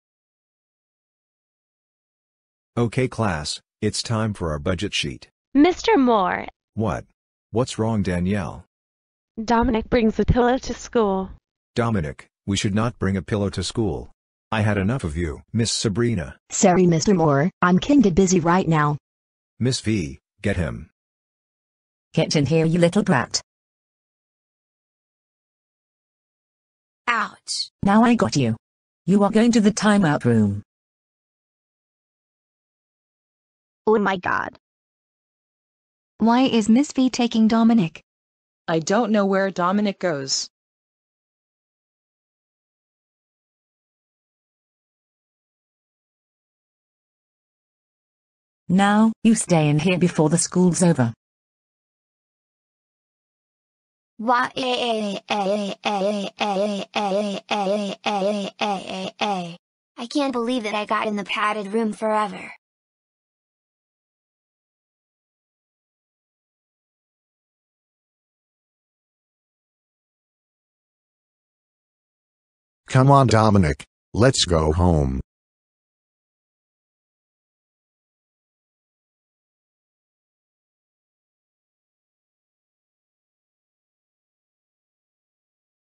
Time for me to go to the padded timeout room!
A Video Of
GoAnimate

Oh no! @jaxanimationsx, @GoTubeYT, @RackAnimate, @BlackboyTheGoAnimator, @andrewanimations1, @TheAnimatorDude5, @JHopReloadedGoAnimate, @MapleAnimations, @Cewyah, @papermariofan, @your-mom-plays, @AfricanVulture, @TheVincentMottola, and more GoAnimate users, help me! I'm stuck in the padded room! Let me out! It's... It's so hot in here!